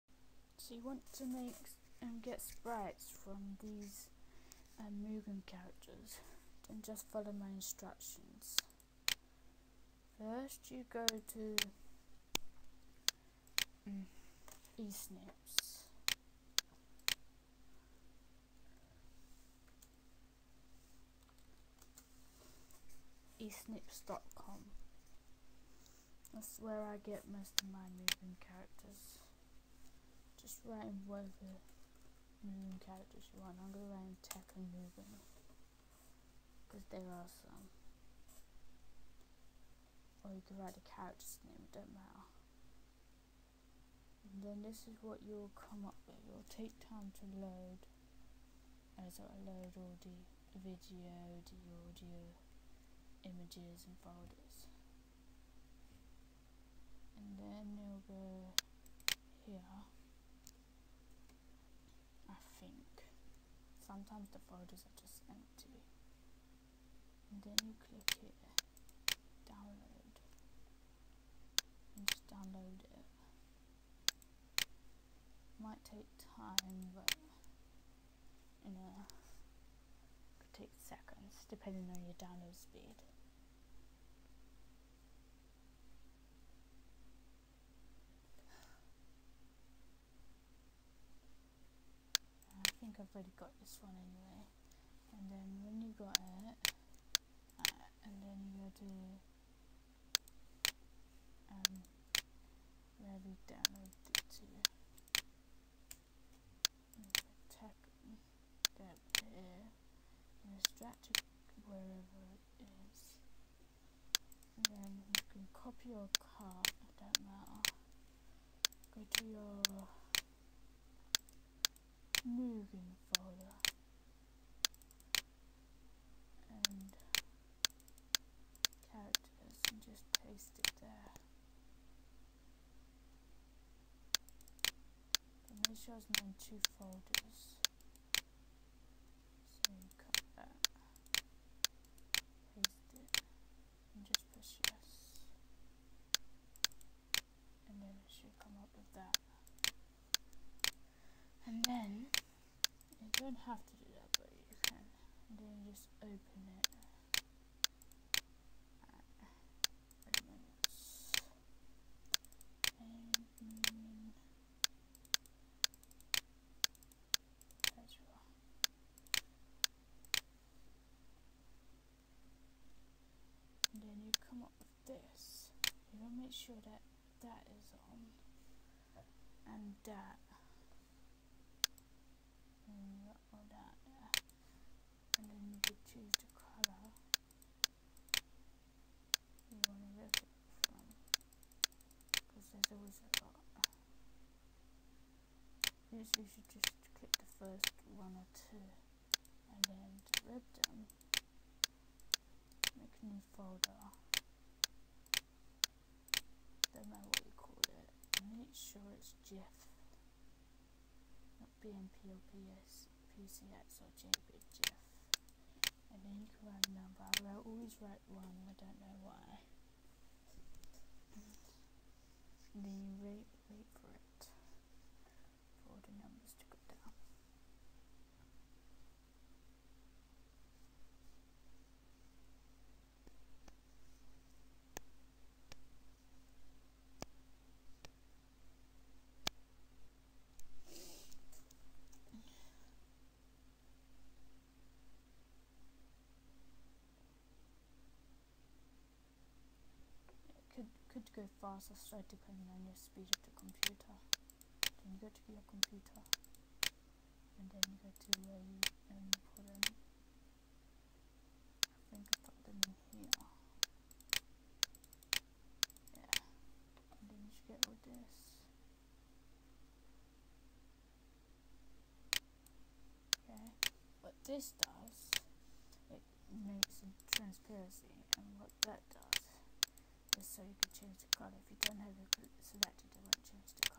How to convert Mugen characters to sprites
this here tutorial tells you how to convert Mugen character pictures to Sprites. Its simple really. And at the end i forgot to mention at the beginning about what the two buttons were for so its at the end ehehehe.